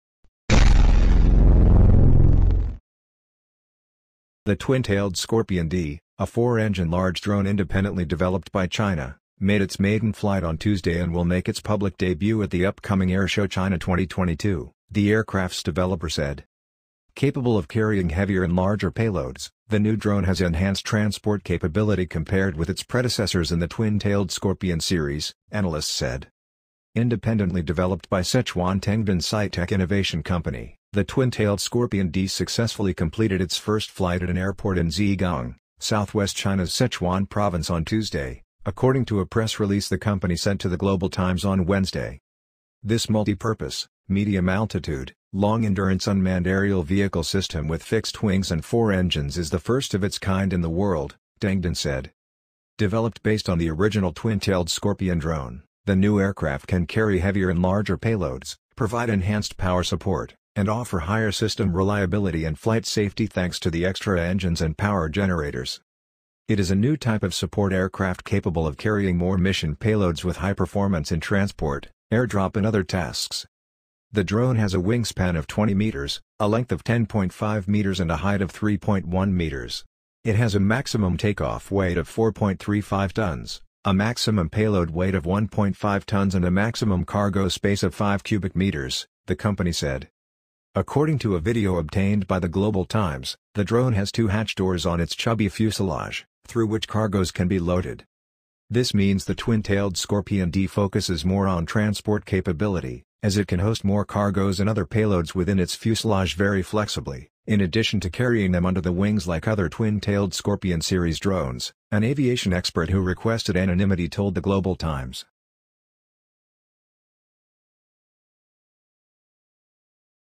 With Four-Engine: China New Most Advanced Large Military Drone Test Flight
The Twin-Tailed Scorpion D, a four-engine large drone independently developed by China, made its maiden flight on Tuesday and will make its public debut at the upcoming Airshow China 2022